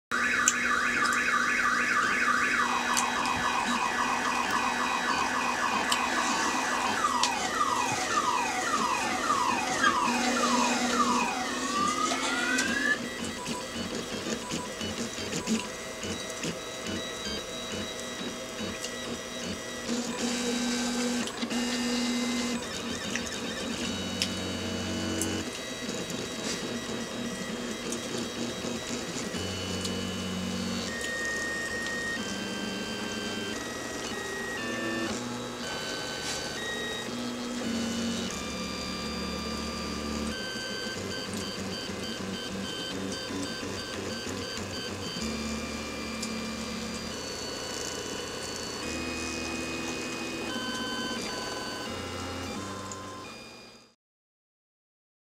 PrintrBot Jr ABS Print Test
Testing the heated bed upgrade on the PrintrBot Jr.